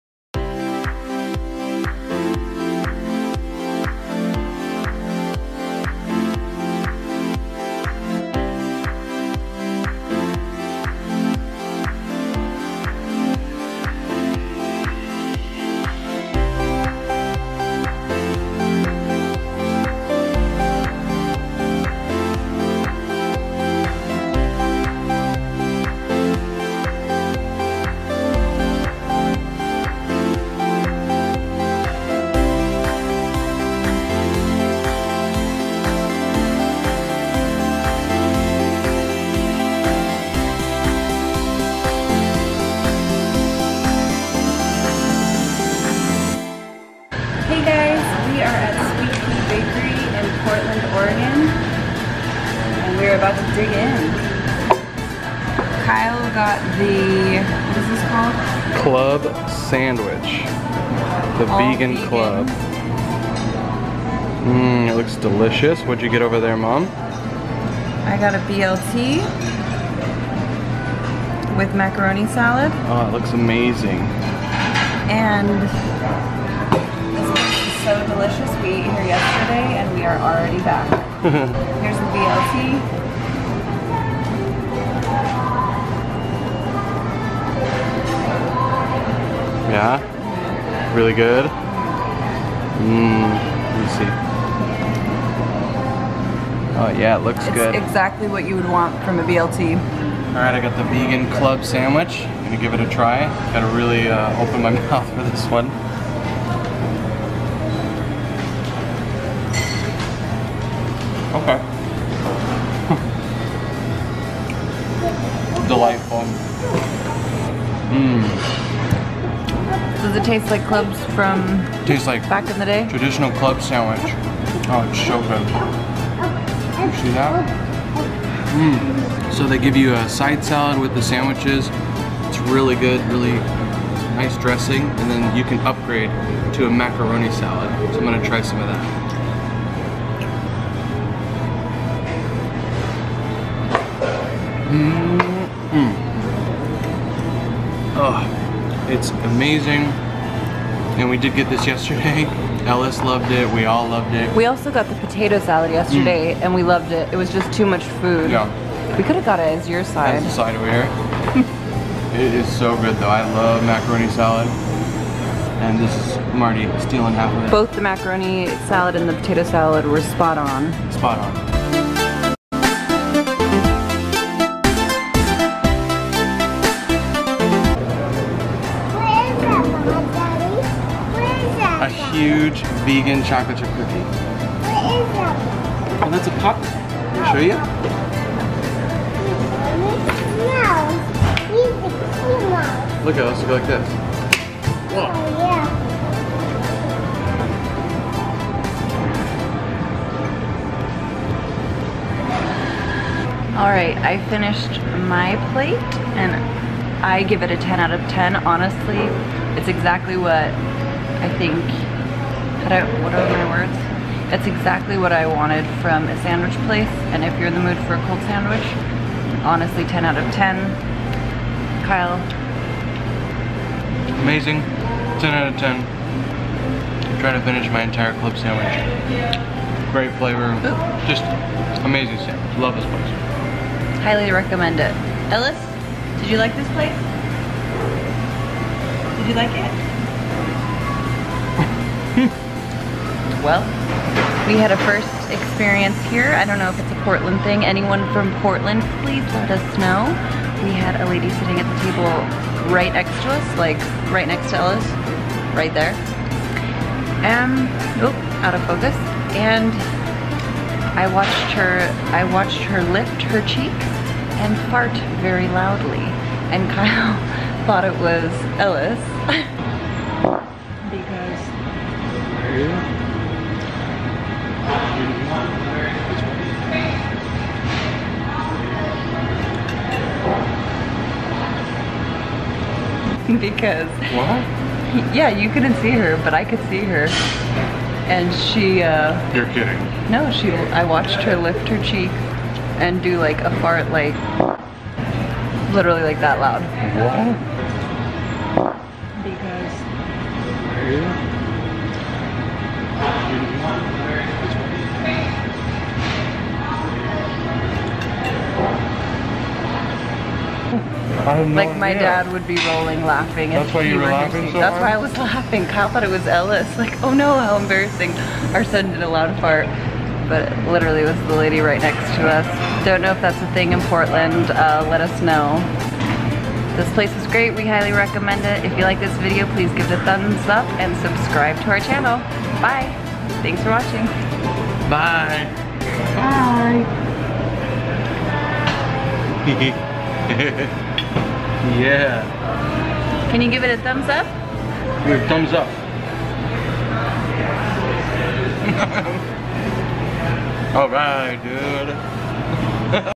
Sweet Pea Bakery | Portland, Oregon
Join us while we eat at Sweat Pea Bakery and catch a customers FART on camera! LOL Not joking!